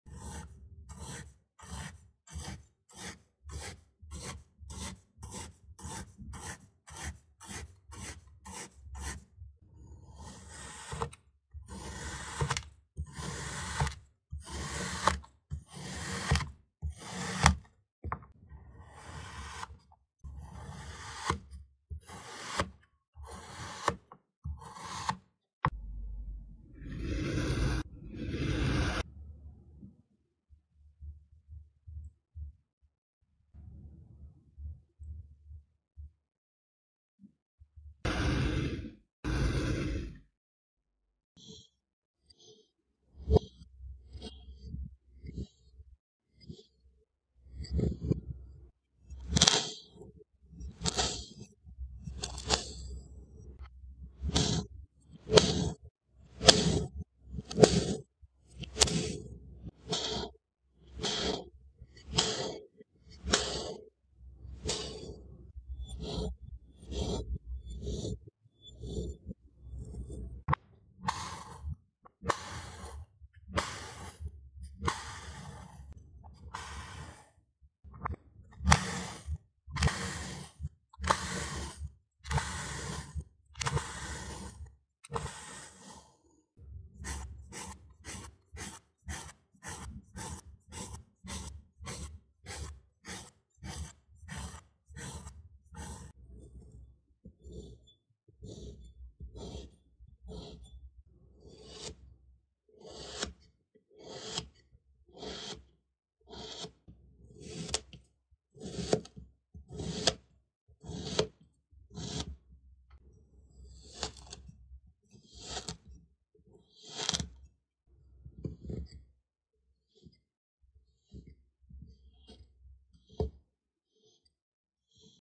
Very Satisfying and Relaxing Kinetic Sand, Crunchy Sand drop and squish # 22
Crunchy Sand cutting, colorful kinetic sand, drop and squish
For your relaxing after a tiring day  and help you fall asleep.